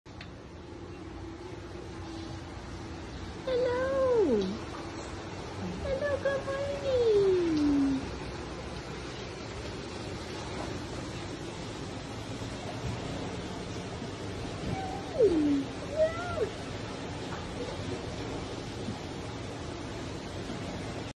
Giant otters do synchronised swimming routine | SWNS
This is the adorable moment giant otters did a synchronised swimming routine as their keeper greeted them in the morning.

The footage, taken by zookeeper Monica Richards, shows the giant otters at Los Angeles Zoo and Botanical Gardens swimming in joyful loops to greet her.

The video was first posted online on September 8.